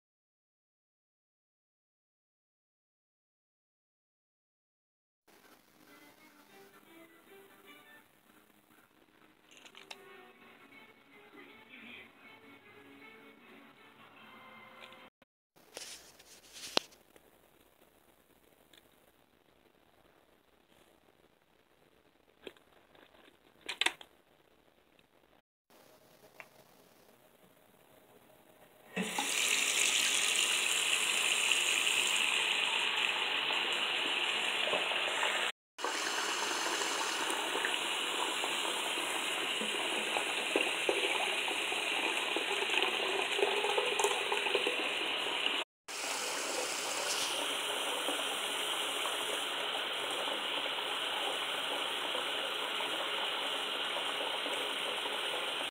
Some Torture You've All Been Waiting For (STINK BUG)
Kept finding stink bugs around the house, so I decided to capture one in a bottle...it stayed alive for 2days. I found a couple more and put them in the bottle as well, along with the other one. Added a little water...maybe they'll suffocate or drown. They ended up living for 3days until I shook the bottle so vigorously heads and wings popped off. Now here we are again, I captured one and wondered if it would survive in HOT🔥 water. here's what happens..